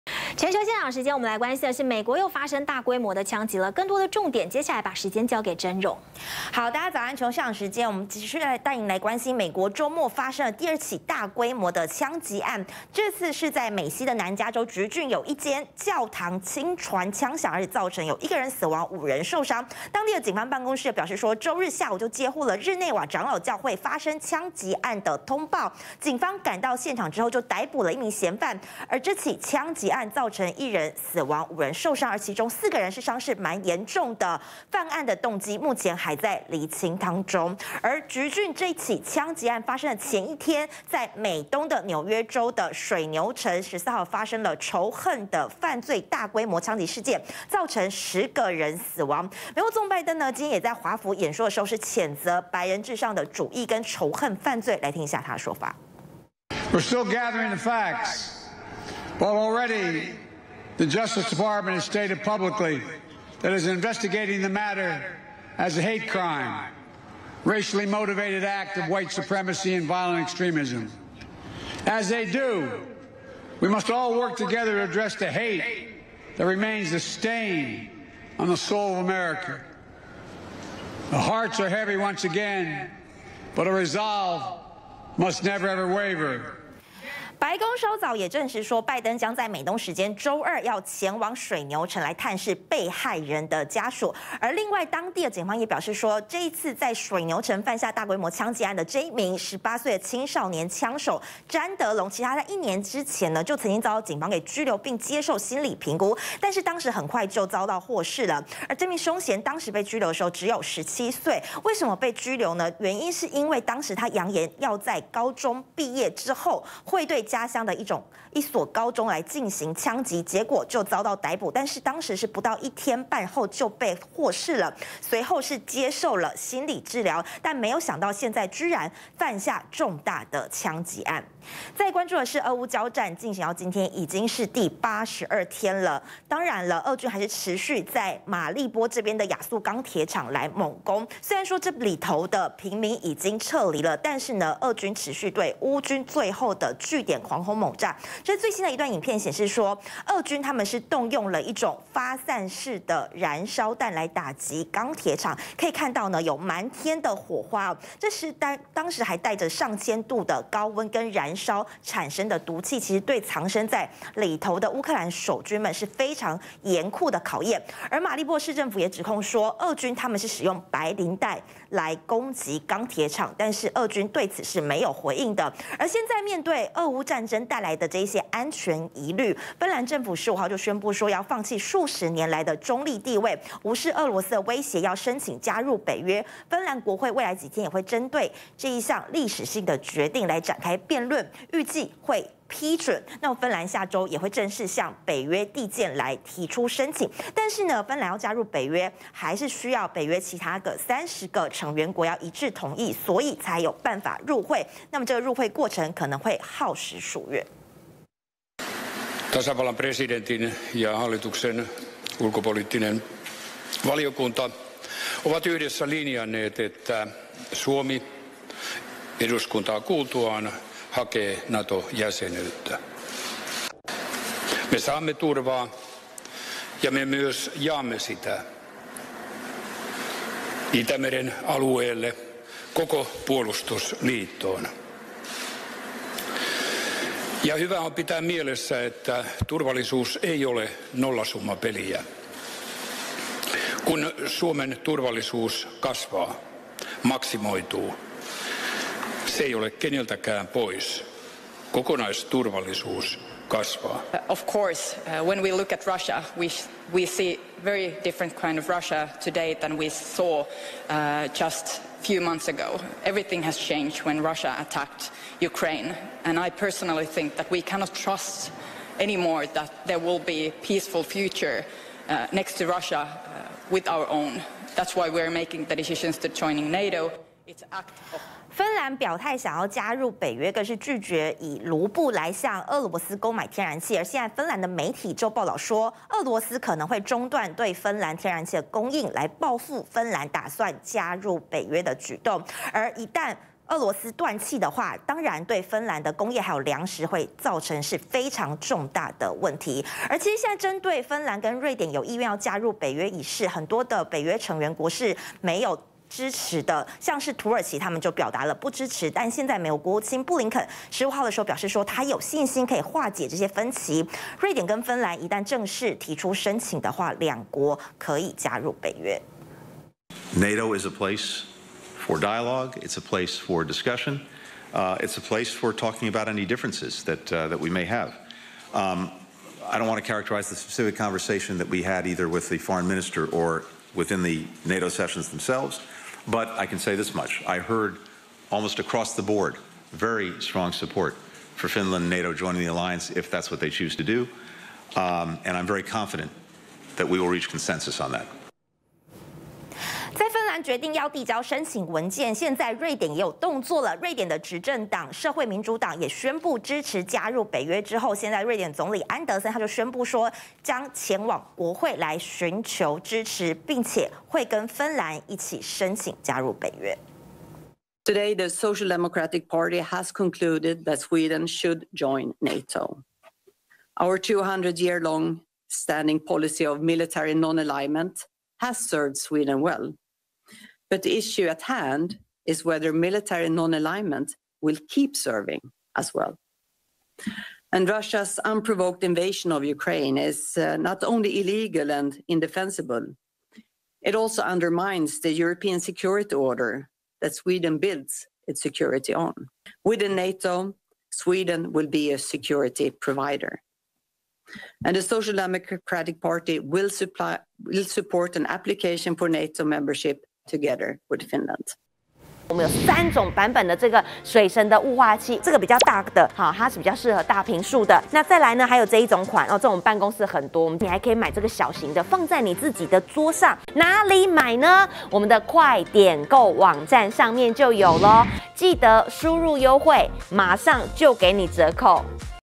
【全球現場】水牛城大規模槍擊 拜登譴責白人至上主義｜報復芬蘭要北約 俄恐"斷氣"報復
00:00 南加州橘郡教堂槍擊已知1死5傷 嫌犯被捕
00:39 水牛城大規模槍擊 拜登譴責白人至上主義
02:12 亞速鋼鐵廠遭燃燒彈轟炸 漫天火花如雨落
02:58 芬蘭宣布申請加入北約 國會將辯論
04:38 報復芬蘭要北約 俄恐"斷氣"報復
05:01 土國反對瑞典芬蘭入北約 美國務卿:有信心取得共識
05:50 執政黨支持 瑞典宣布與芬蘭申請加入北約